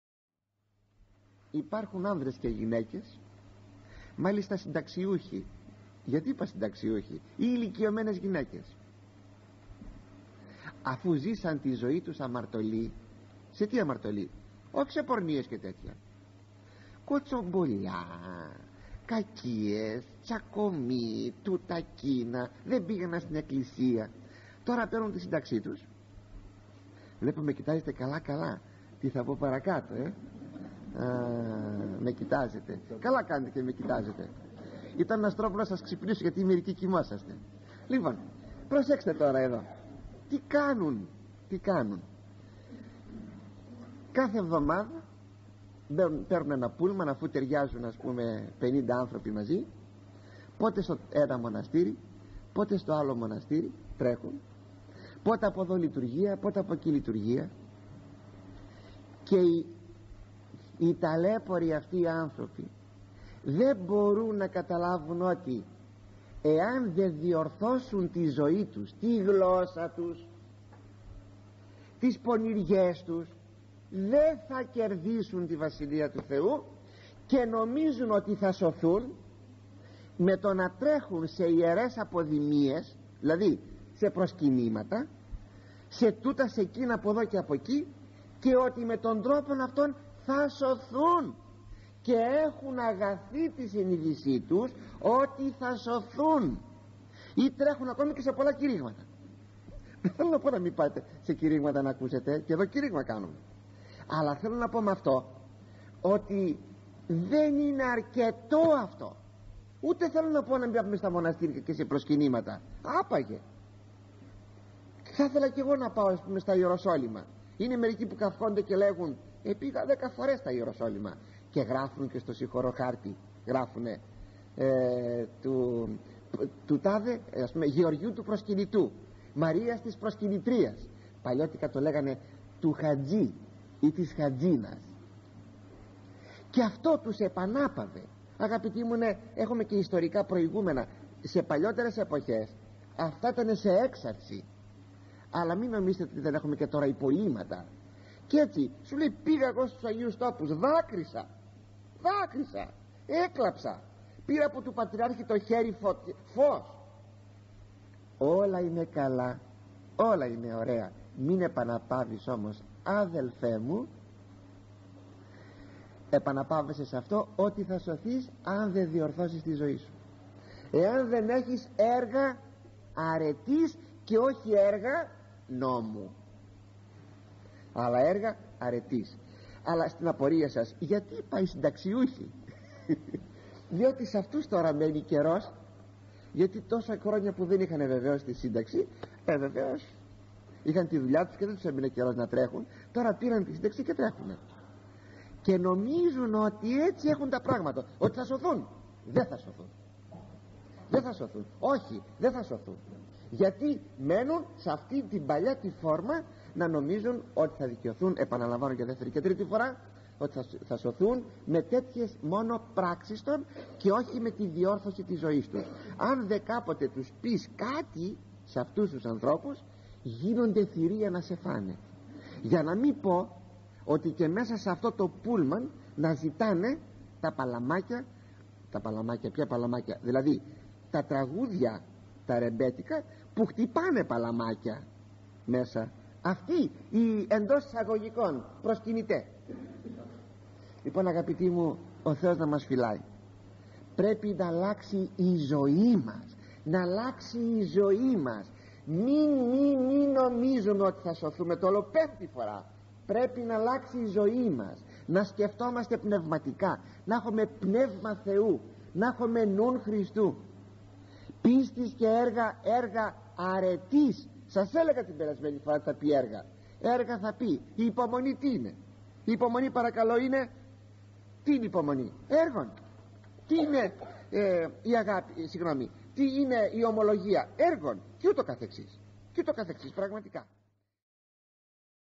Χωρίς διόρθωση της ζωής σωτηρία δέν υπάρχει.
► Κατηγορία :
" Καθολική Ἐπιστολή Ἁγίου Ἰακώβου τοῦ Ἀδελφοθέου "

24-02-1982

Κεφ 2, 20-26

Σχέσις πίστεως καί ἔργων.(Μέρος Β΄).

►Όλες οι ομιλίες της Κατηγορίας :
" Καθολική Ἐπιστολή Ἁγίου Ἰακώβου τοῦ Ἀδελφοθέου " εδώ ⬇️